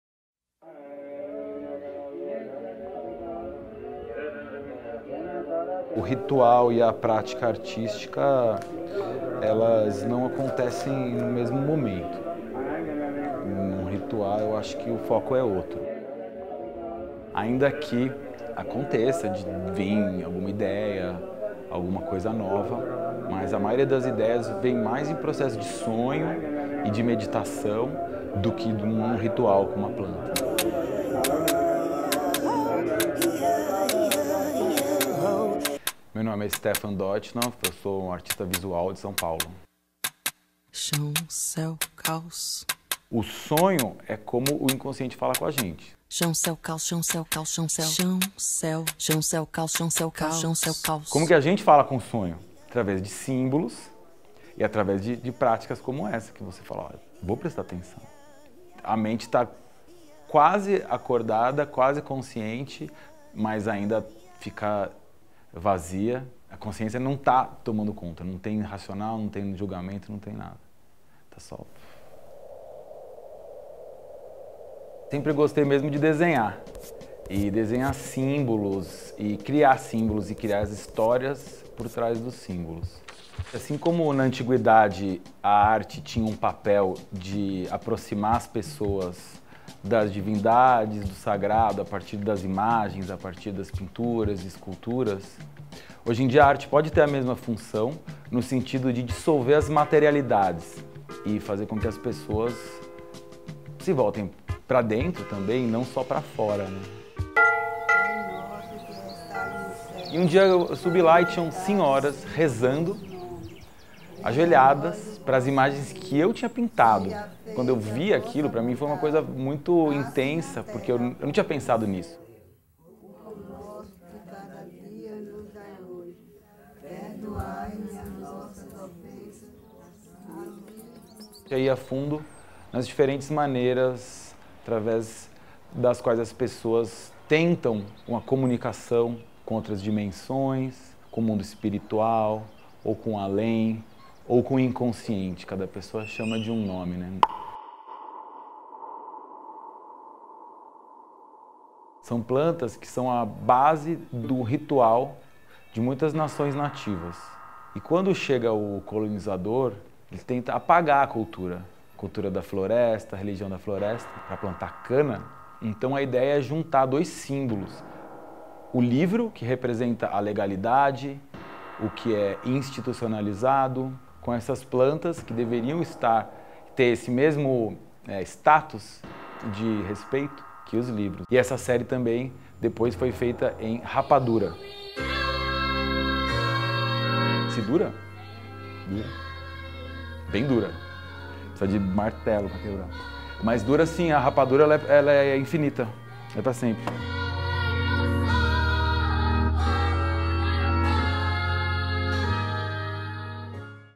Stephan Doitschinoff, um artista devoto
Para Stephan Doitschinoff os símbolos e rituais sagrados são materiais de pesquisa para o seu trabalho. Há mais de uma década o artista mergulha em práticas de diferentes religiões e recria um universo sagrado próprio, que reúne de ex-votos à plantas psicoativas. Em entrevista para a Trip ele abre seu ateliê para falar de criação, fé e inconsciente.

O trabalho do artista está exposto na Galeria Janaína Torres, em São Paulo, até o dia  7/12/19.